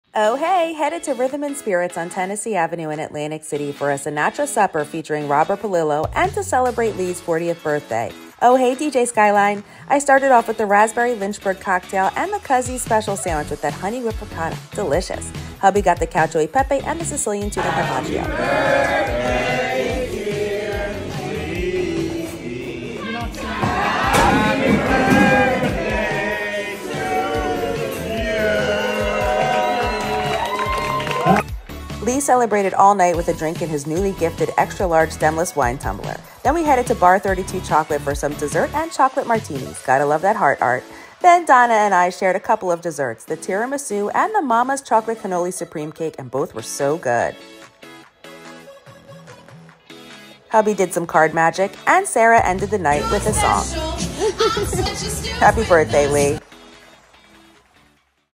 Dinner at Rhythm & Spirits + Dessert at Bar32 Chocolate in Atlantic City | Tennessee Avenue
ohheyyy Headed to Rhythm & Spirits { @rhythmandspiritsac } on Tennessee Avenue in Atlantic City for a Sinatra Supper featuring Robert Polillo and to celebrate Lee’s 40th birthday. ohheyyy DJ Skyline { @dj.skyline }.

🍹I started off with the Raspberry Lynchberg cocktail, and the Cuzzie’s Special sandwich with that honey whipped ricotta, delish! Hubby got the Cacio E Pepe and the Sicilian Tuna Carpaccio.

🎉Lee celebrated all night with a drink in his newly gifted extra large stemless wine tumbler.

🍫We headed to Bar32 Chocolate for some dessert and Chocolate Martini’s - gotta love that heart art, then Donna and I shared a Tiramisu and Mama’s Chocolate Cannoli Supreme Cake - both were soooo good!

🎤Hubby did some card magic and Sara ended the night with a song. Happy Birthday Lee!